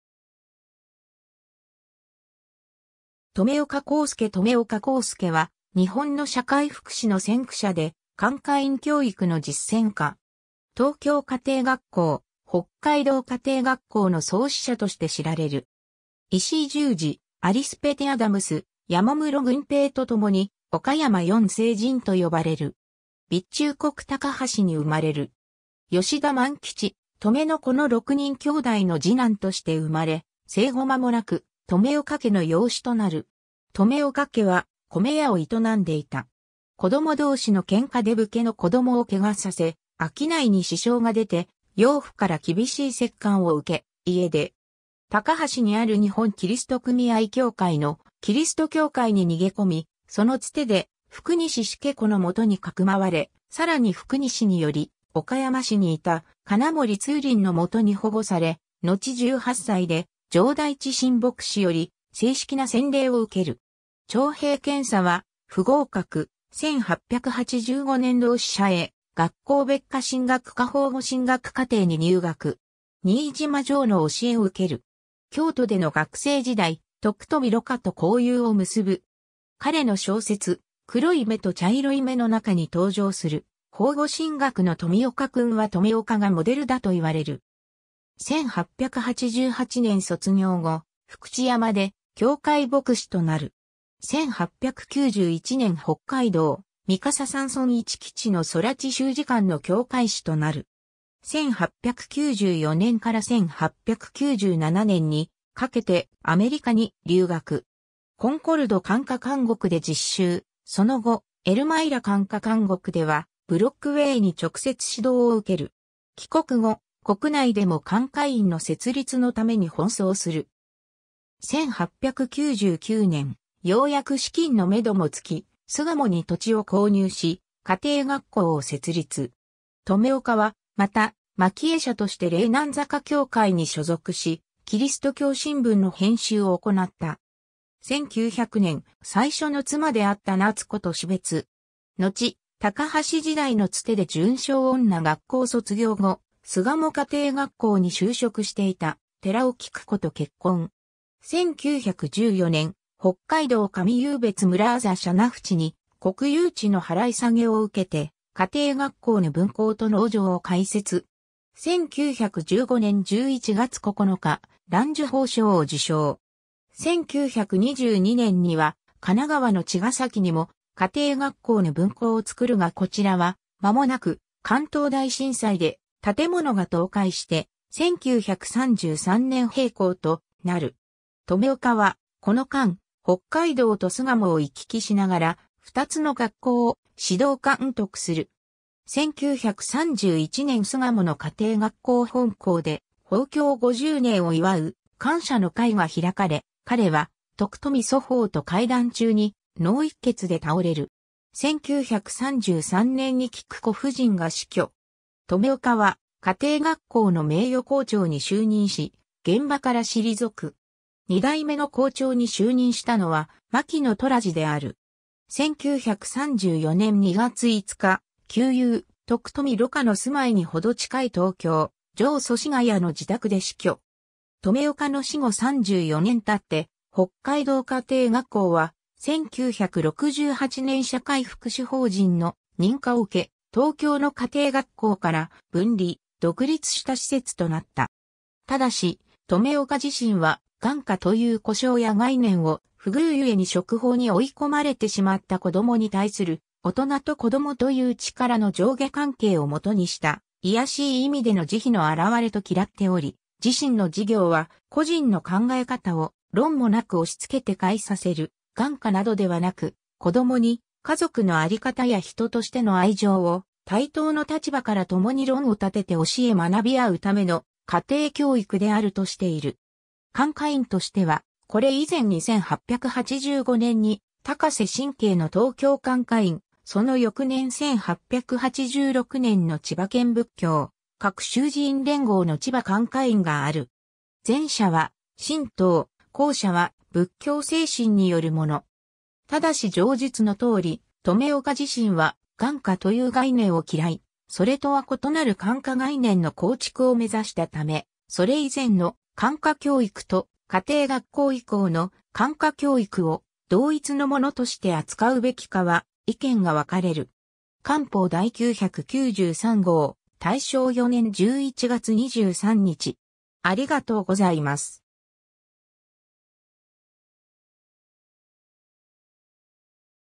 留岡幸助
留岡幸助 留岡 幸助（とめおか こうすけ、1864年4月9日〈元治元年3月4日〉 - 1934年〈昭和9年〉2月5日）は、日本の社会福祉の先駆者で、感化院（現在の児童自立支援施設のこと）教育の実践家。
東京家庭学校、北海道家庭学校の創始者として知られる。
石井十次、アリス・ペティ・アダムス、山室軍平とともに「岡山四聖人」と呼ばれる。
 備中国高梁（現・岡山県高梁市）に生まれる。
吉田万吉、トメの子の6人兄妹の次男として生まれ、生後まもなく、留岡家の養子となる。
留岡家は、米屋を営んでいた。
子供同士の喧嘩で武家の子供を怪我させ、商いに支障が出て、養父から厳しい折檻を受け、家出。
高梁にある日本基督組合教会のキリスト教会に逃げ込み、その伝で福西志計子の元に匿われ、さらに福西により岡山市にいた金森通倫の元に保護され、のち18歳で上代知新牧師より正式な洗礼を受ける。
 徴兵検査は不合格、1885年（明治18年）同志社英学校別科神学科邦語神学課程に入学。
新島襄の教えを受ける。
京都での学生時代、徳富蘆花と交友を結ぶ。
彼の小説『黒い眼と茶色い眼』の中に登場する「邦語神学の富岡君」は留岡がモデルだといわれる。
1888年（明治21年）卒業後、福知山で教会牧師となる。
 1891年（明治24年）北海道三笠山村市来知（いちきしり）の空知集治監の教誨師となる。
1894年（明治27年）から1897年（明治30年）にかけてアメリカに留学。
コンコルド感化監獄で実習、その後、エルマイラ感化監獄ではブロックウェーに直接指導を受ける。
 帰国後、国内でも感化院（家庭学校）の設立のために奔走する。
1899年（明治32年）、ようやく資金の目処もつき、巣鴨に土地を購入し、家庭学校を設立。
留岡は、また牧会者として霊南坂教会に所属し、「基督教新聞」の編集を行った。
 1900年（明治33年）、最初の妻であった夏子と死別。
のち高梁時代の伝で順正女学校卒業後、巣鴨家庭学校に就職していた寺尾きく子と結婚。
 1914年（大正3年）、北海道上湧別村字社名淵（しゃなぶち）に国有地の払い下げを受けて、家庭学校の分校と農場を開設。
1915年（大正4年）11月9日、藍綬褒章を受章。
 1922年（大正11年）には神奈川の茅ヶ崎にも家庭学校の分校を作るがこちらはまもなく関東大震災で建物が倒壊して、1933年（昭和8年）閉校となる。
留岡はこの間、北海道と巣鴨を行き来しながら、二つの学校を指導監督する。
 1931年（昭和6年）巣鴨の家庭学校本校で、奉教五十年を祝う感謝の会が開かれ、彼は徳富蘇峰と会談中に脳溢血で倒れる。
1933年（昭和8年）にきく子夫人が死去。
留岡は家庭学校の名誉校長に就任し、現場から退く。
二代目の校長に就任したのは、牧野虎次である。
1934年（昭和9年）2月5日、旧友・徳冨蘆花の住まいに程近い東京・上祖師谷の自宅で死去。
 留岡の死後34年経って北海道家庭学校は、1968年（昭和43年）社会福祉法人の認可を受け、東京の家庭学校から分離、独立した施設となった。
 ^ ただし留岡自身は「感化」という呼称や概念を「不遇ゆえに触法に追い込まれてしまった子どもに対する、大人と子どもという力の上下関係を元にした、卑しい意味での慈悲のあらわれ」と嫌っており、自身の事業は「個人の考え方を論も無く押し付けて変えさせる『感化』などではなく、子どもに家族の在り方や人としての愛情を対等の立場から共に論を立てて教え学び合うための『家庭教育』である」としている。
 ^ 感化院としては、これ以前に1885年に高瀬真卿の東京感化院、その翌年1886年の千葉県仏教各宗寺院連合の千葉感化院がある。
前者は神道、後者は仏教精神によるもの。
（それ以前にも池上雪江の活動も挙げられる）ただし上述の通り留岡自身は「感化」という概念を嫌い、それとは異なる感化概念の構築を目指したため、それ以前の「感化教育」と家庭学校以降の「感化教育」（家庭教育ないしは児童自立支援教育）を同一のものとして扱うべきかは意見が分かれる。
 ^ 『官報』第993号、大正4年11月23日。